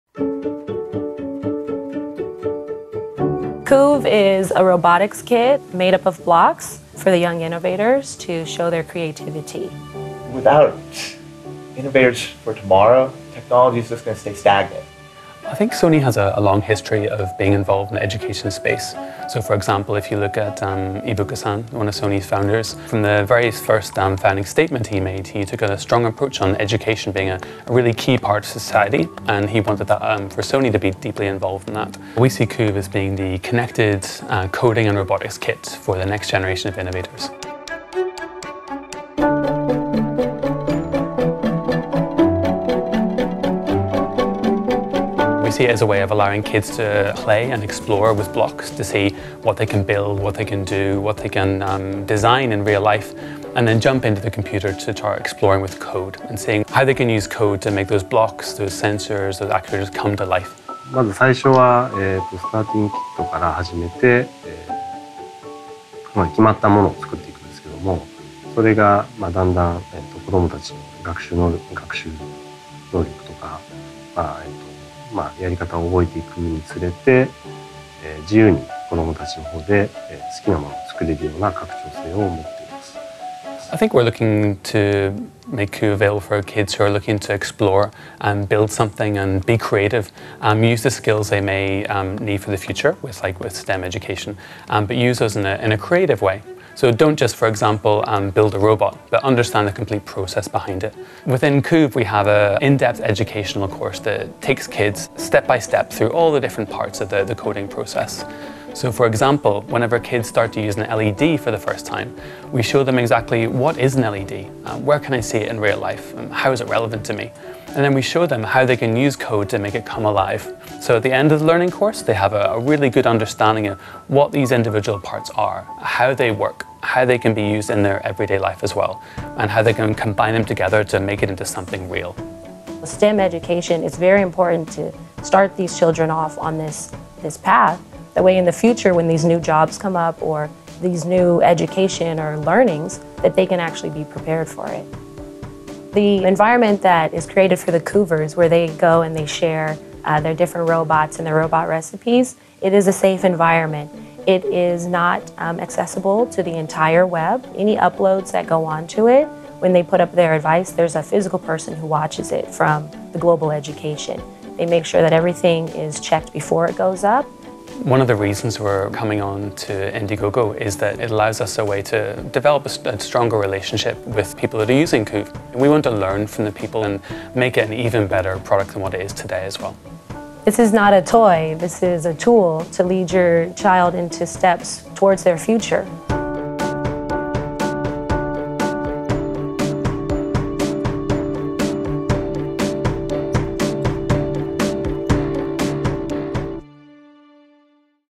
KOOV: The coding & robotics kit for the next generation of young innovators
KOOV is a robotics and coding educational kit made up of blocks, sensors, motors, actuators, and a companion app that teaches children core concepts about design, coding and robotics.